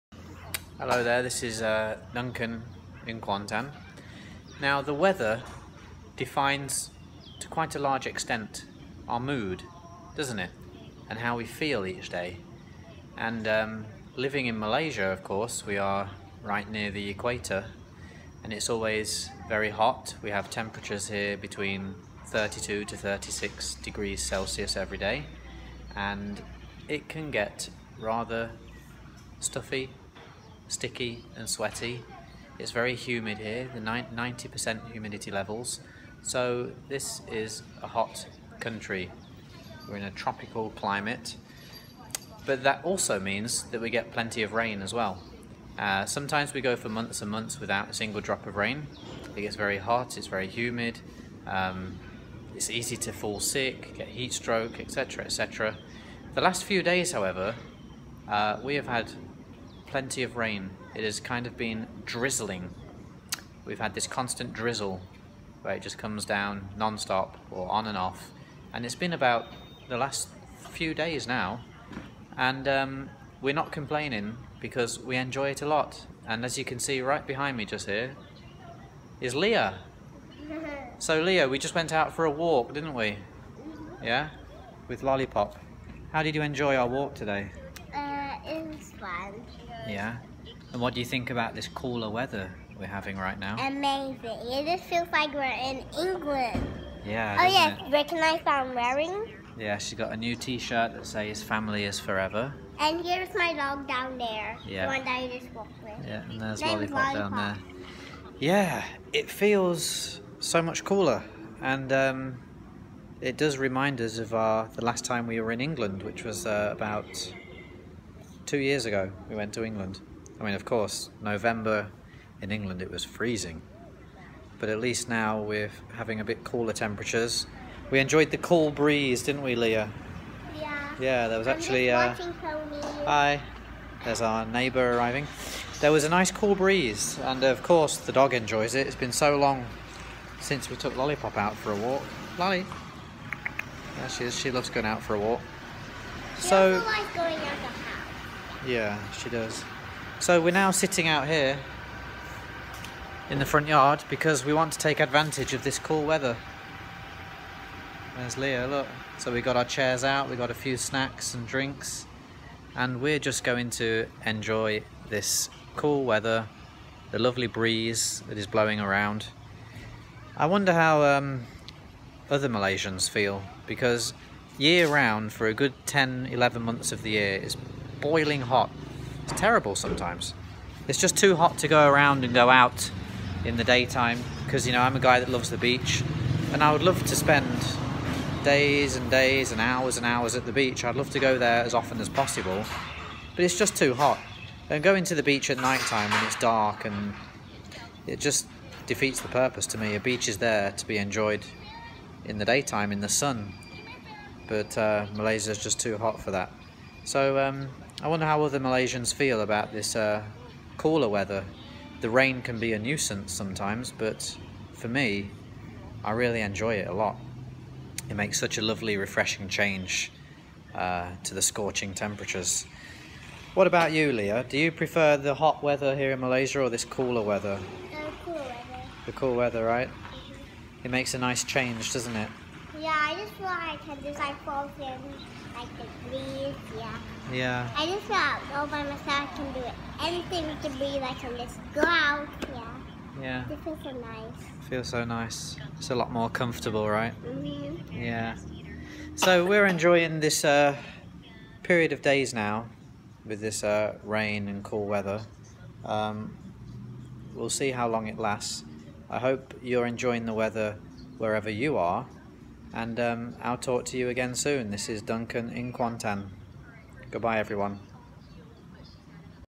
Malaysia’s (slightly) Cooler Weather
Just enjoying the drizzle and cooler temperatures while it lasts!